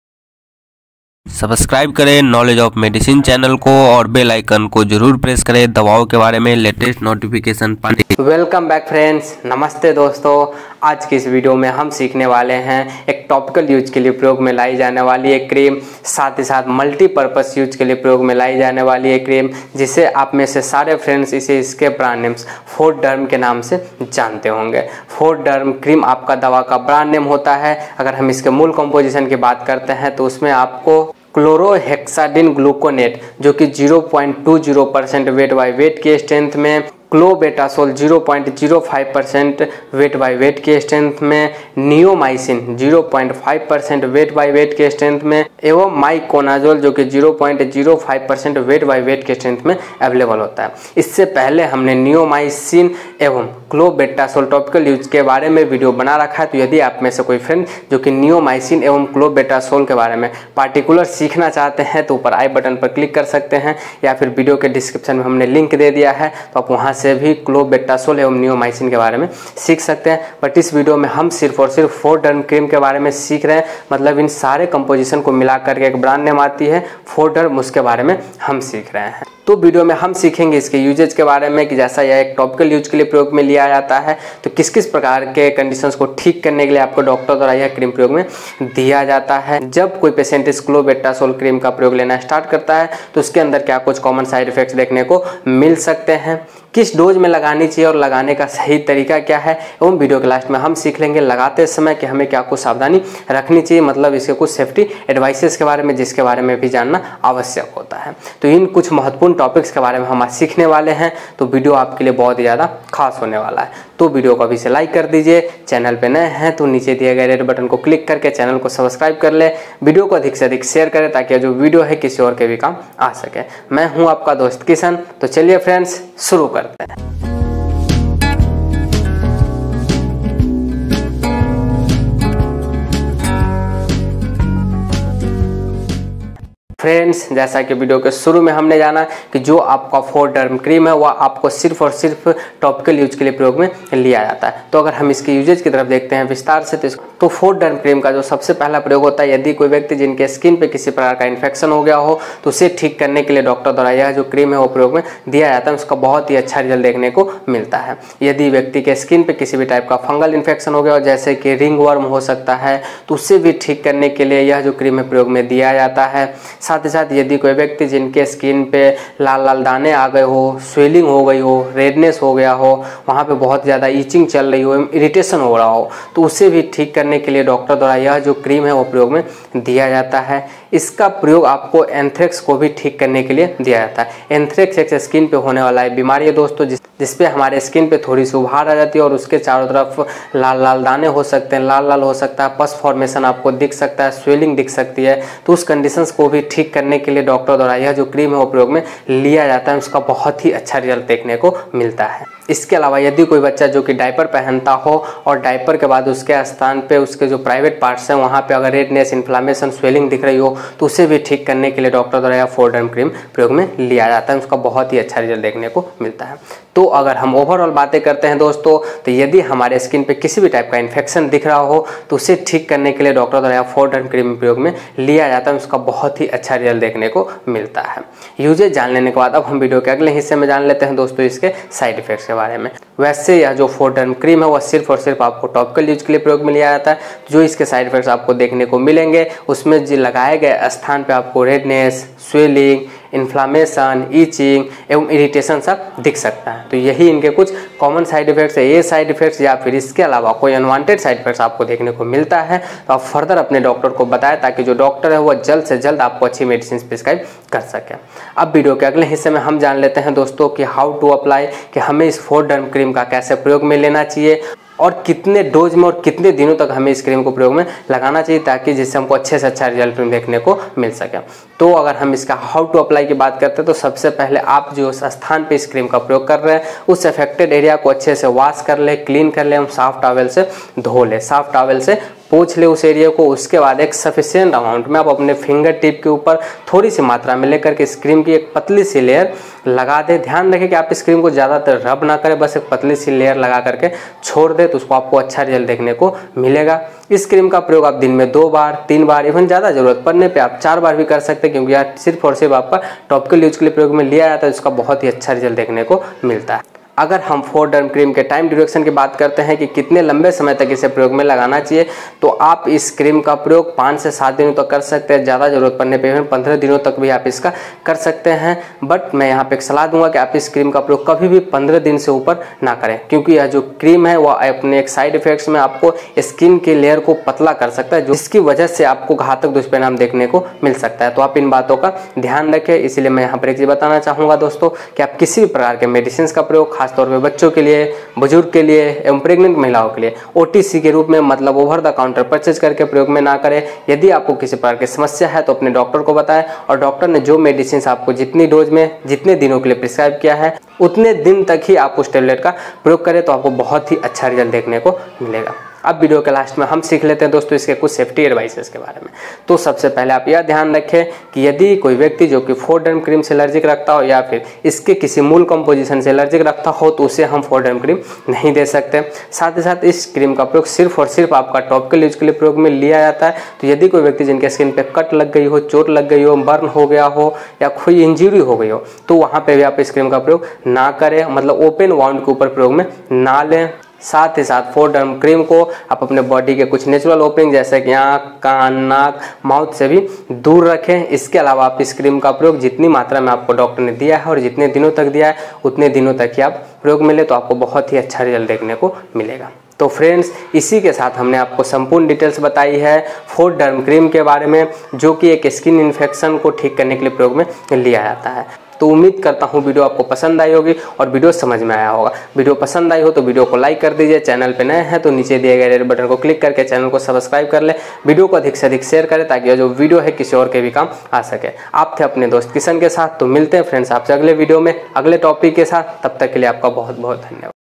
Fourderm cream |Fourderm cream review (Clobetasol propionate, Neomycin sulphate, Miconazole Nitrate)
Hi
   ⚠️🚨 Serious Warning ⚠️

   👉 Friends  Don't take/give self medications . This video is only for knowledge purpose. 👈

   ️🚨 गंभीर चेतावनी ️

   👉 दोस्तो स्वयं दवा न लें / न दें । यह वीडियो केवल ज्ञान के उद्देश्य के लिए है।👈

In This Video Contains :-

(1)  "FourDerm " Kya  hai ??
(2)  " FourDerm" Similar Brand's Name
(3)  "FourDerm " Uses in Details
(4)  " FourDerm" Common Dosage for Adults
(5)  " FourDerm" Side effects ( Common & Rare )
(6)  """""" Precautions & Warnings

      [ Knowledge Of Medicine ]

Playlist Link's :-